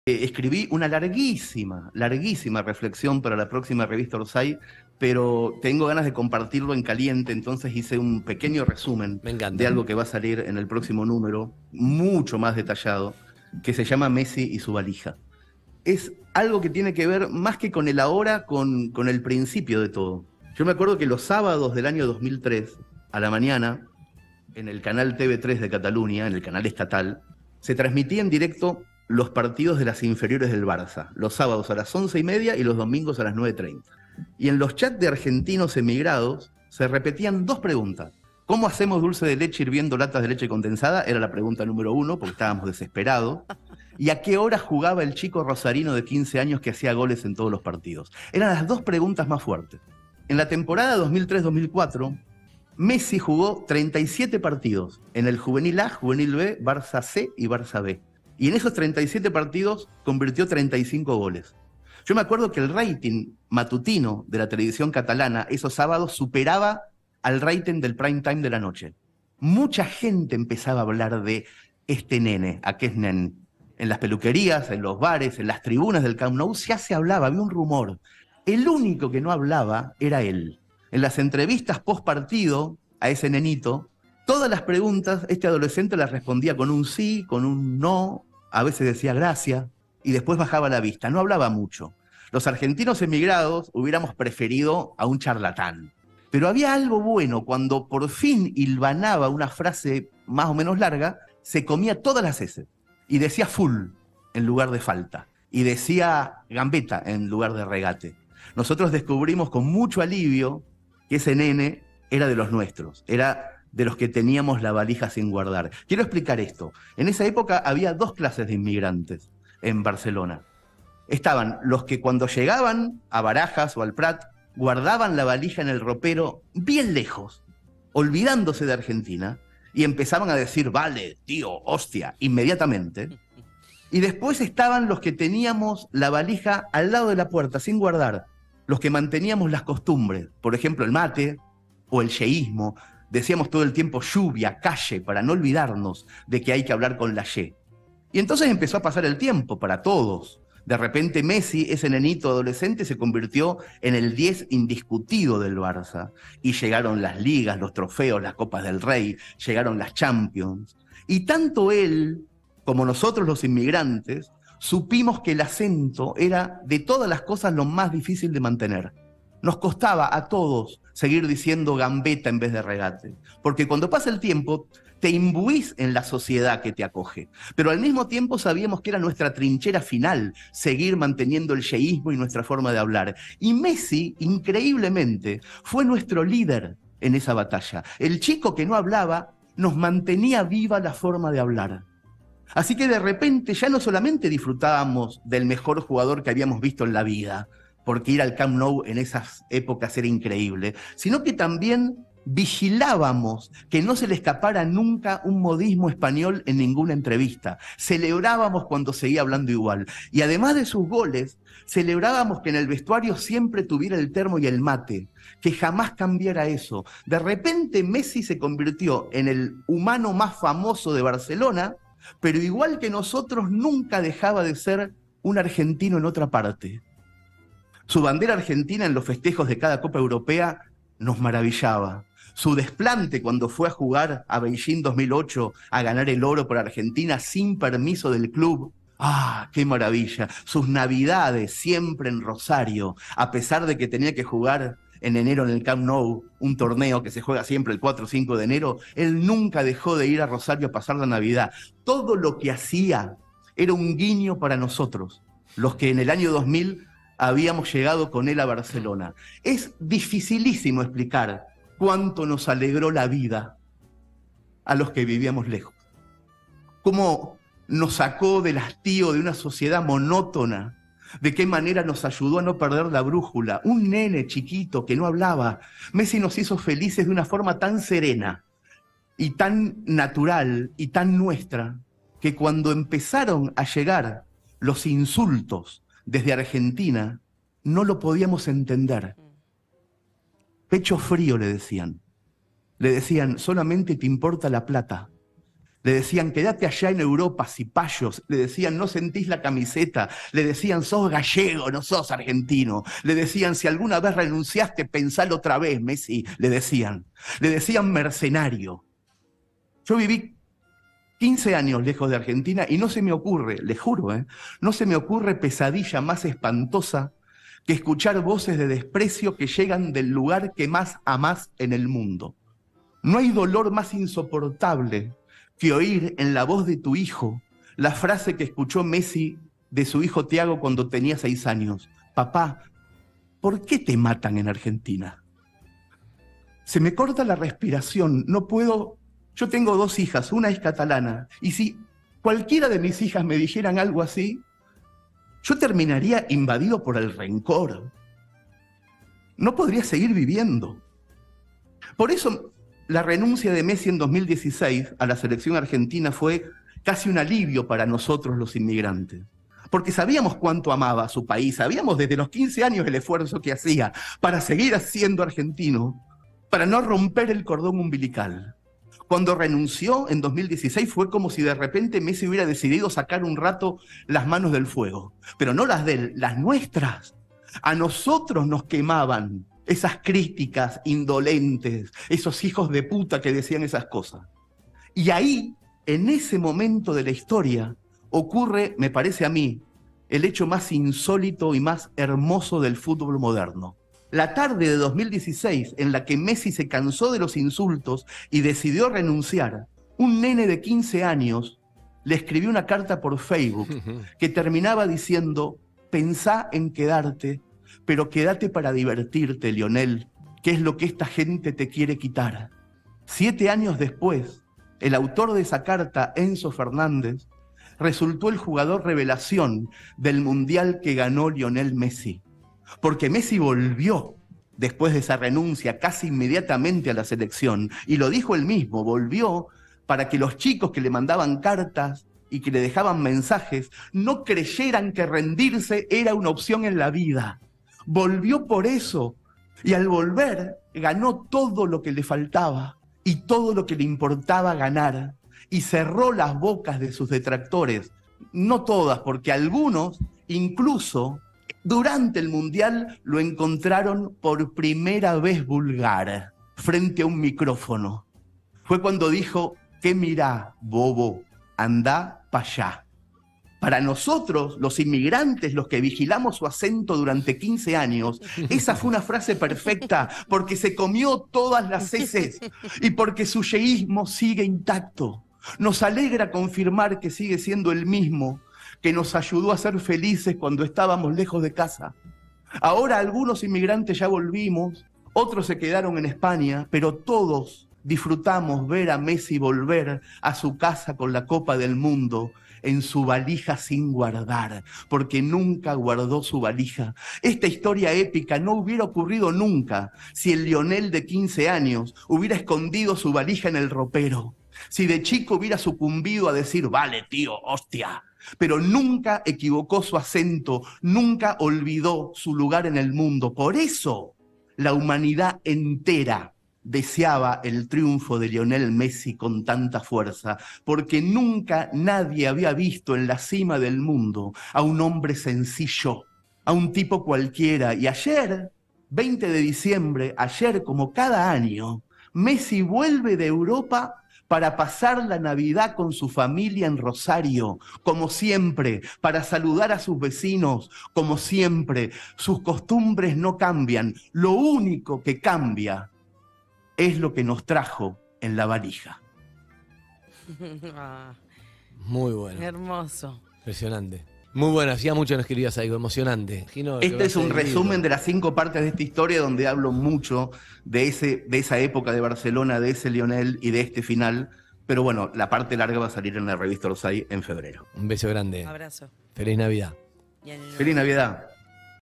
Un adelanto de «La valija de Lionel», una crónica que saldrá en la próxima revista Orsai
La nueva Revista Orsai sale en febrero y tiene una crónica larga y serena sobre el nuevo Lionel, que ya no es un perro. Esta mañana intenté resumir la estructura de esa crónica en nueve minutos de radio. Les comparto ese resumen en audio y texto y los invito a reservar la revista, que será histórica,